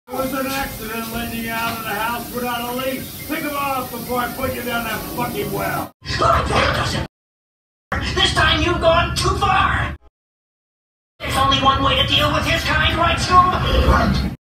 Young Shaggy And Young Scooby Stands Up To William Murphy
This Is My Second William Murphy Video.